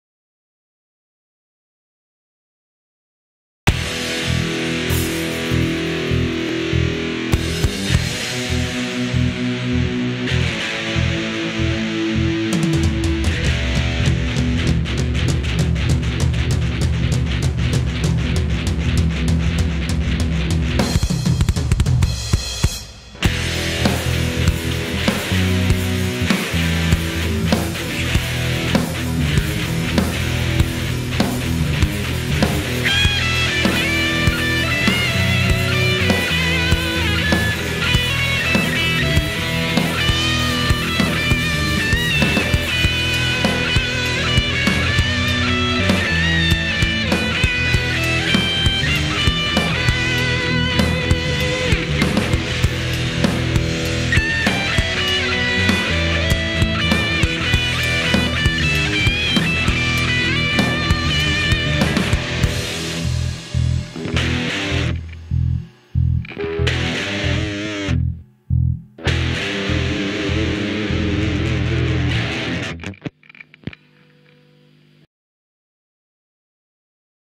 SWOLA84
SWOLA84, Sunday with Ola#84, SWOLA#84
Sunday with Ola riff Challenge #84
Gear used:  Westone Raider Guitar, Peavey Valveking, Jackson Concert Bass.